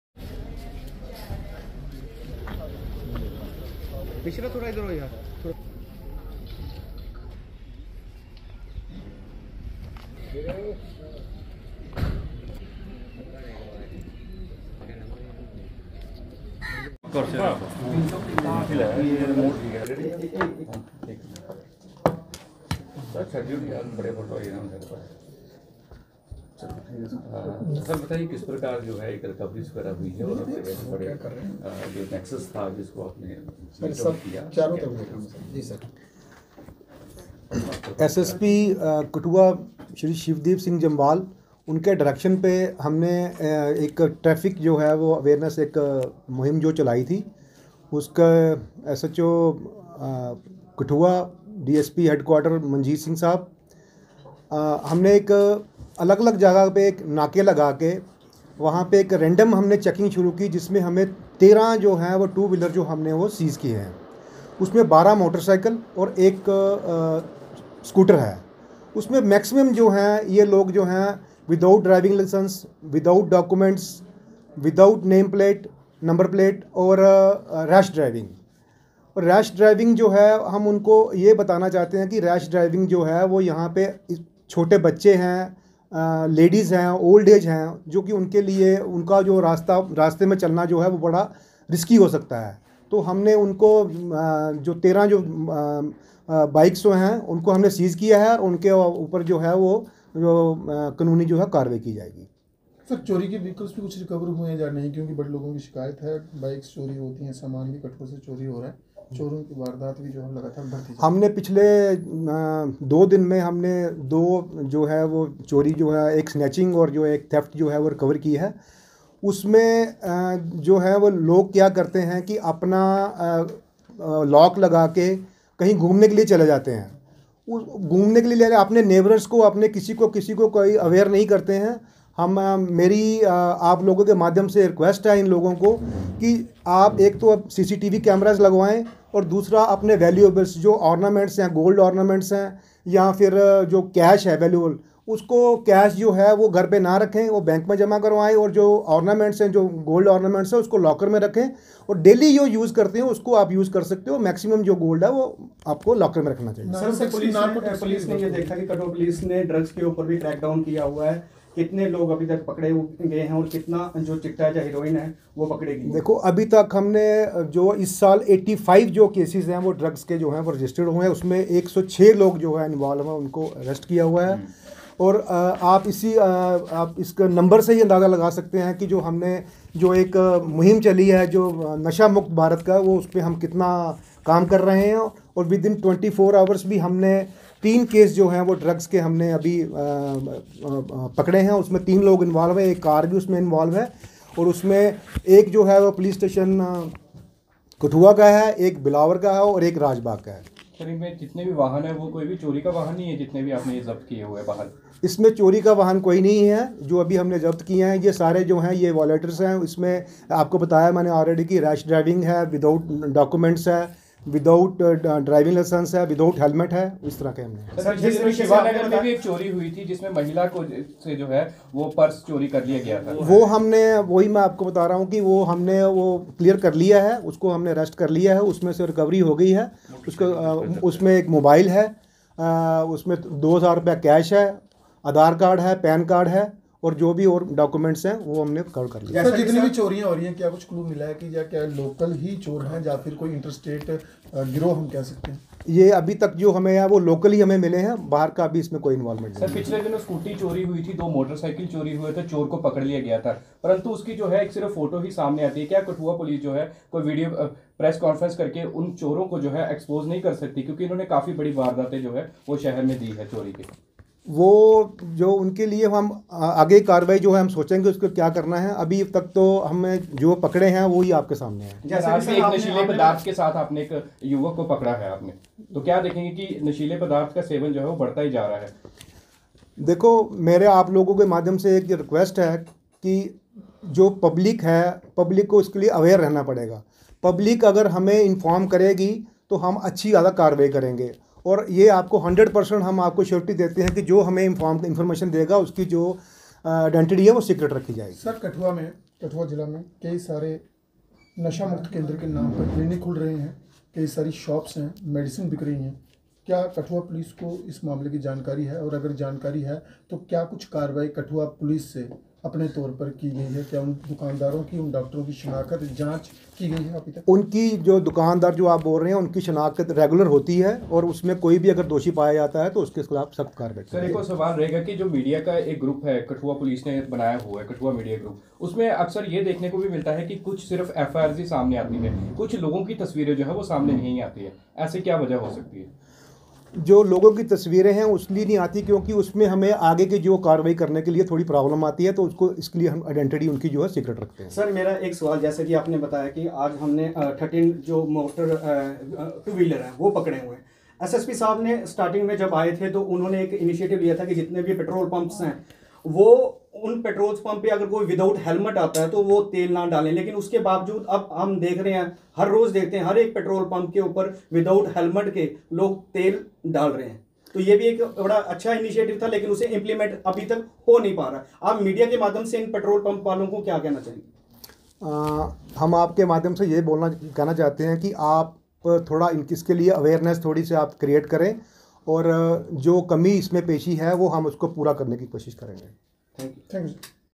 Additional SP Kathua Paramjeet Singh breif Media person regarding Theft Cases
Additional SP Kathua Paramjeet Singh breif Media person regarding Theft Cases Solved/Recovery of Heroin like narcotics/13 Vehicles Seized U/S 207/MV act held at Police Station Kathua